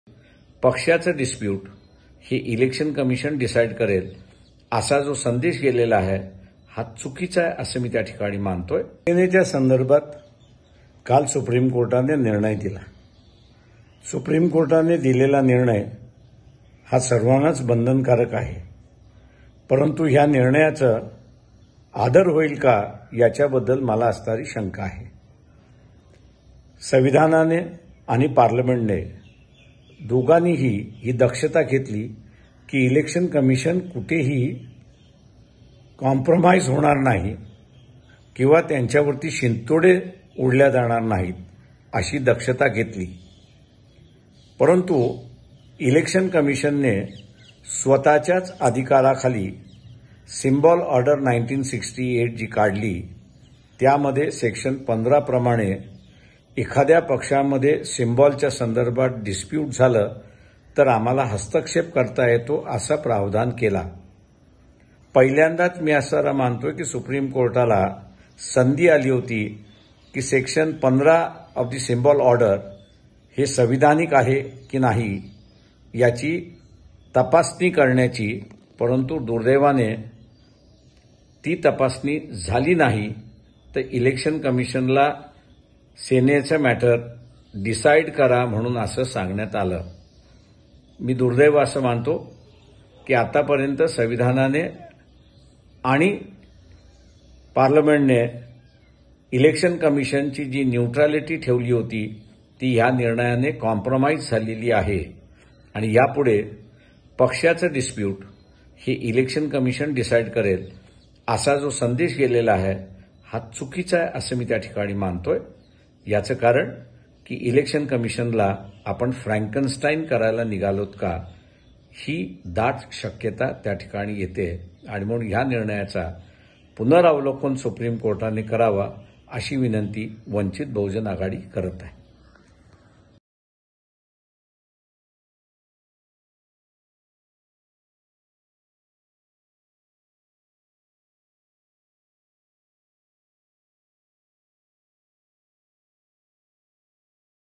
Prakash Ambedkar | सर्वोच्च न्यायालयाच्या निकालामुळे चुकीचा संदेश - प्रकाश आंबेडकर | LetsUpp Marathi
सुप्रीम कोर्टाने मंगळवारी झालेल्या सुनावणीत शिवसेनेच्या निवडणूक चिन्हाबाबत निवडणूक आयोगाला सुनावणी करण्यास मंजुरी दिली आहे. या निकालानंतर अनेक प्रतिक्रिया उमटत आहेत. शिवसेना-शिंदे गटाच्या सुनावणीमुळे सर्वोच्च न्यायलयाने एक चांगली संधी गमावली असल्याची प्रतिक्रिया वंचित बहुजन आघाडीचे अध्यक्ष प्रकाश आंबेडकर यांनी व्यक्त केली आहे.